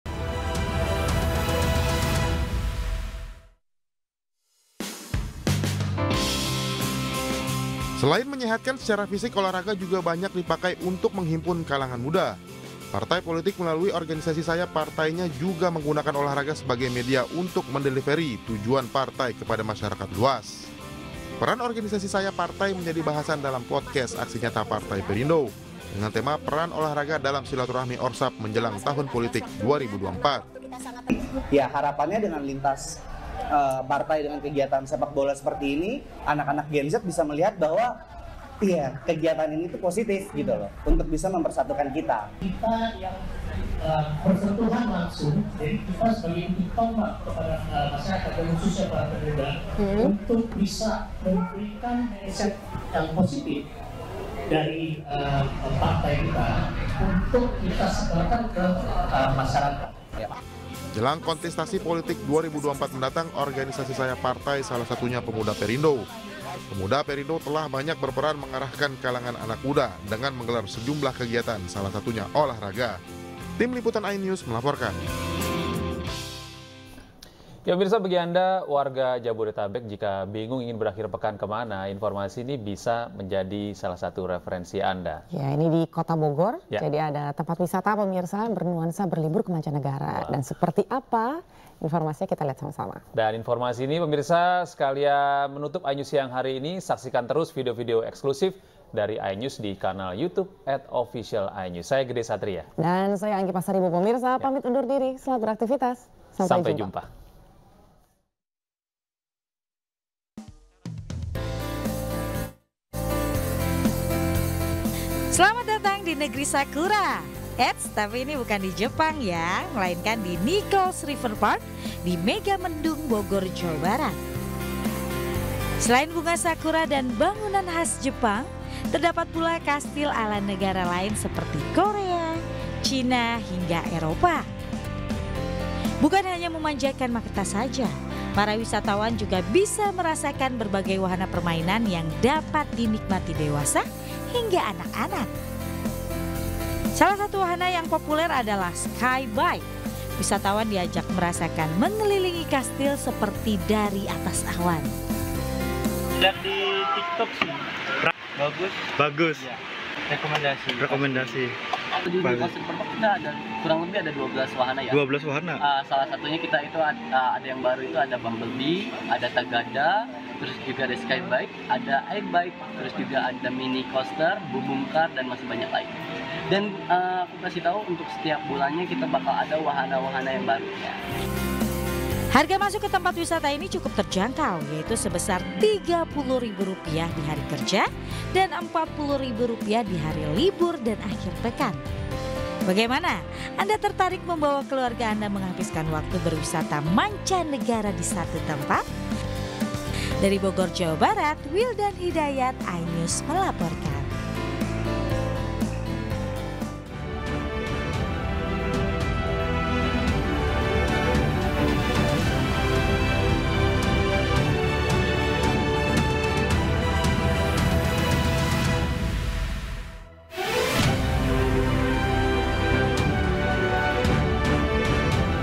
Wisata Lokal Rasa Mancanegara 'Nicole's Castel' Sajikan Wahana Permainan Seru di Bogor
Bagi anda yang ingin berlibur di akhir pekan dengan menikmati sensasi berbeda seperti di Negara Jepang anda bisa berkunjung ke salah satu destinasi wisata di Bogor, "Nicol'es Castel".